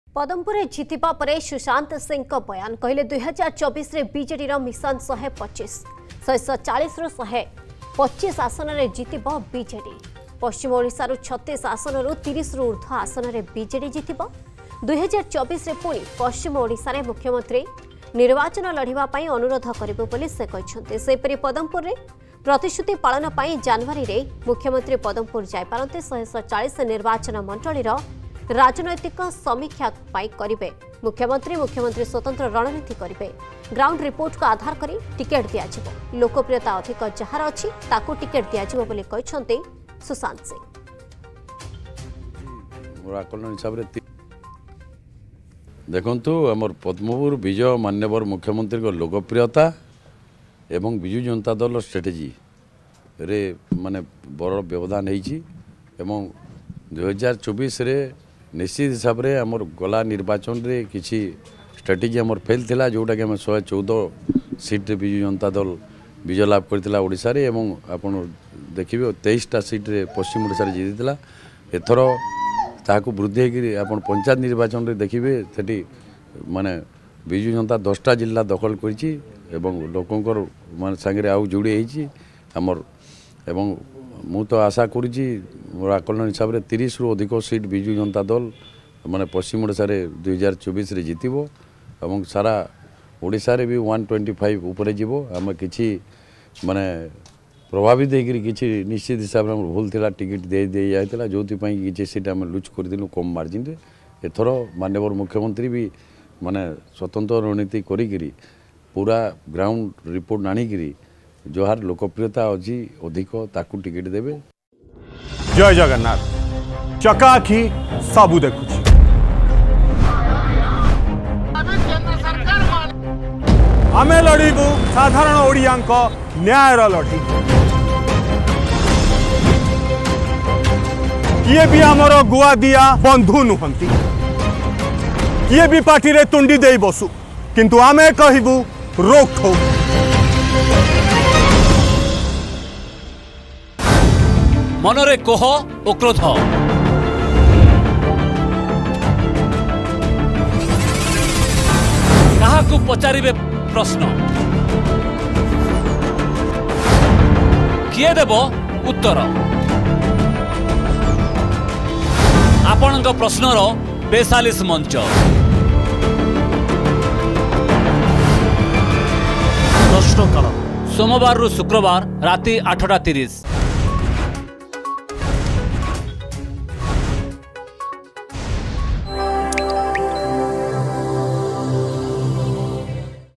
Susanta Singh | BJD Will Win 125 Seats in 2024 Elections
BJD Will Win 125 Seats in 2024 Elections